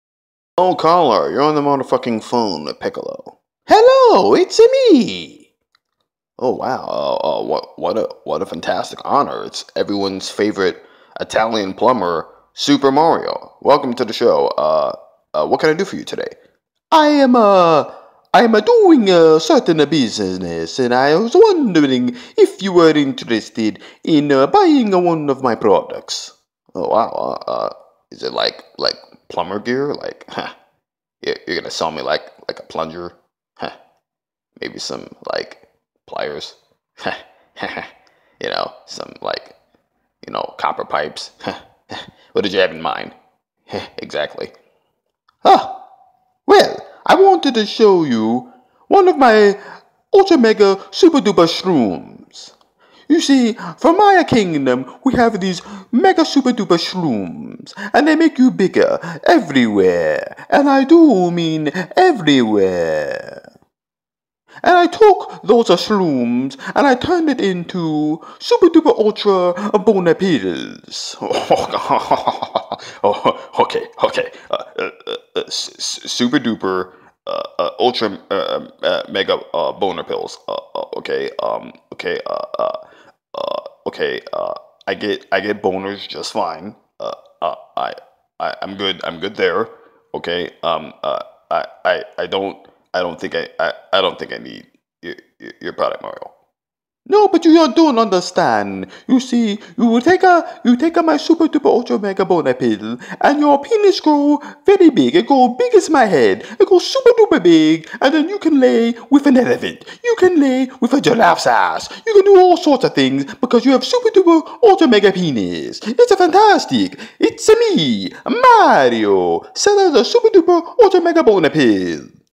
The MP/XLII: Shroom Salesman...
Mario goes pill pushing as he tries to get Piccolo to order some of his Super-Duper Ultra Mega Boner Pills.